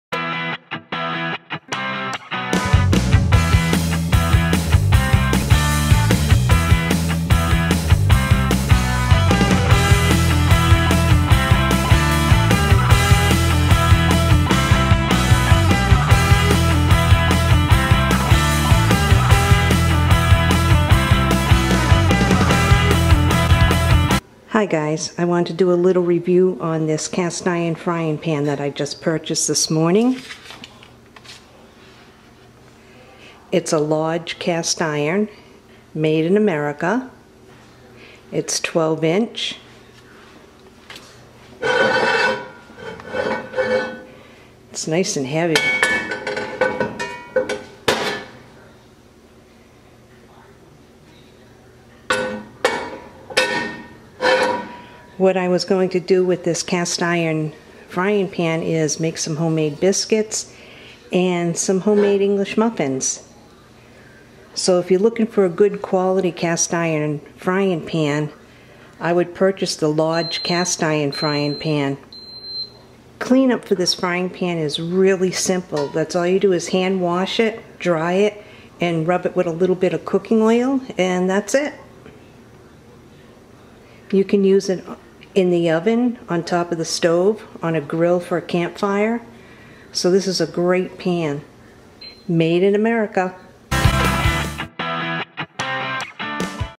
LODGE CAST IRON FRYING PAN/ MADE IN AMERICA/ EPISODE 776/CHERYLS HOME COOKING
This cast iron frying pan from "Lodge" is American made, and perfect for what I need it for. Easy valet up too. very useful. I love this pan. Good quality frying pan.

Checkout my web site.